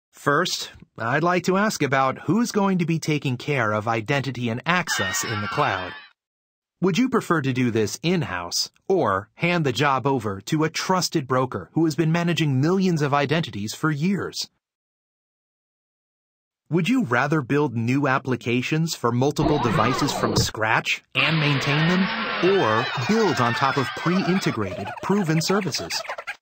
How to Explain a Technology Solution with a 2-Minute Explainer Video
Raising questions about the best use of your I.T. resources to point up key differentiators. Excerpt from Covisint Cloud Enablement series. 2-Minute Explainers produced by Business Information Graphics